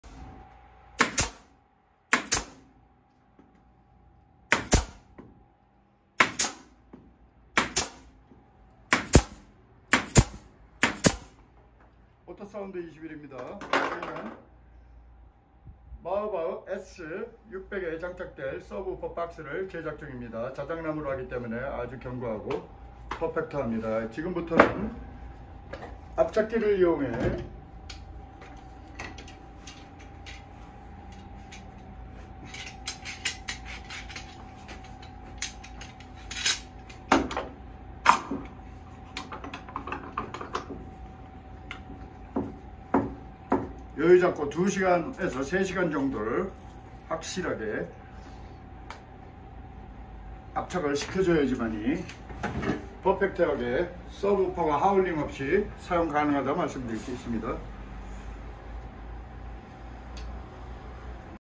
벤츠 마이바흐 S600 2017년식+ 골프백 넣도록 트렁크 확장 공사와 DD 서브우퍼 하이브리드 파워켑 작업.
벤츠 마이바흐 S600 2017년식+ 골프백 넣도록 트렁크 확장 공사와 DD 서브우퍼(280만원)+ 하이브리드 파워켑 30페럿(127만원)작업 사진 및 동영상 입니다.
예약 하시고
방문하신 고객님  전체적으로 소리도 커지고 사운드도 좋아졌지만,  저음 자체도 아주 단단하고 힘있게 나온다고 하시면서 아주 기분좋게 가셨습니다.

•••35년 전통•••
•••대한민국 음질전용 블루투스 작업 NO-1•••
상담문의